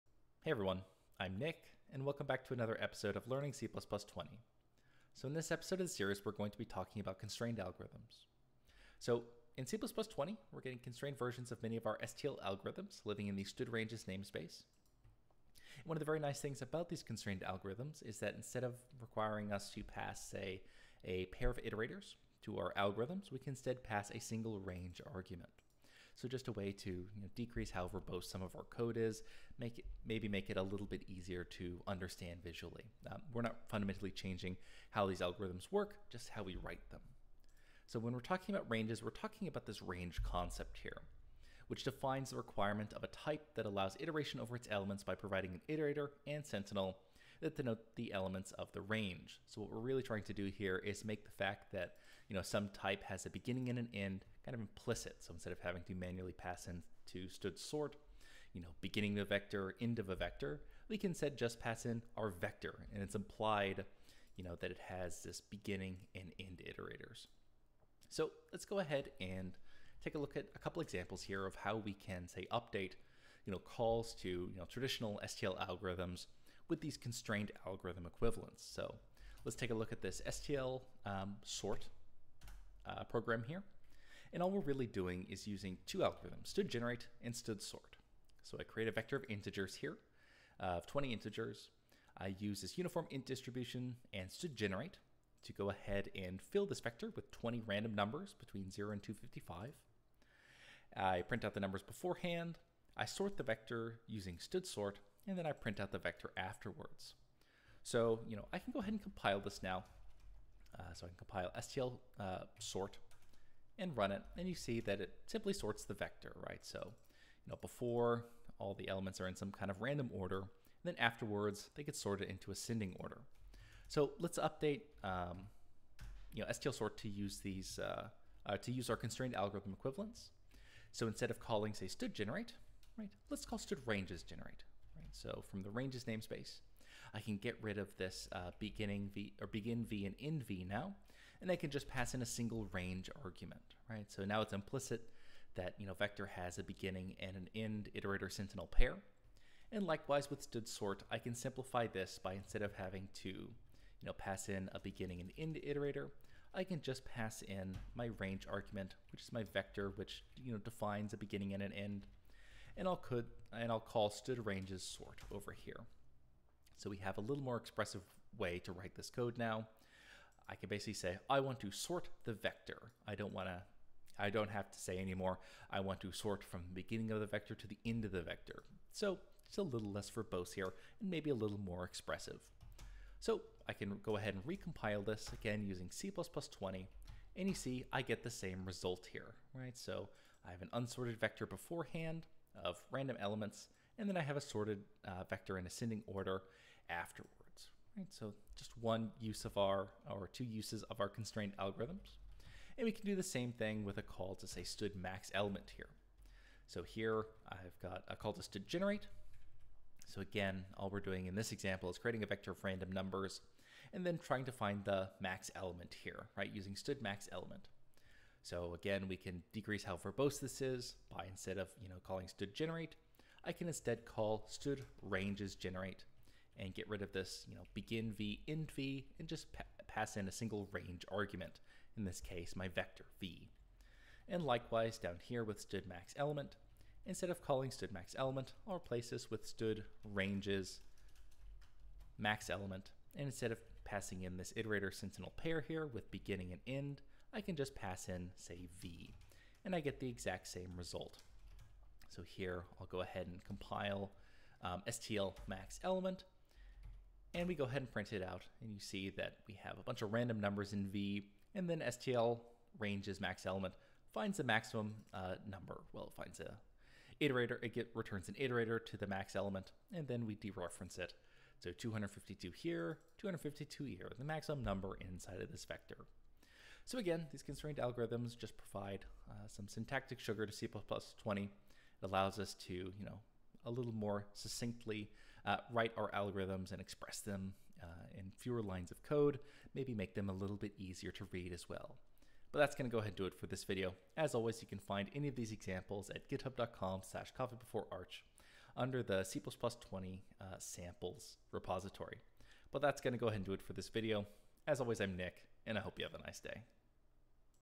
Learning C++20: Constrained Algorithms
In this video we look at constrained algorithms in C++20!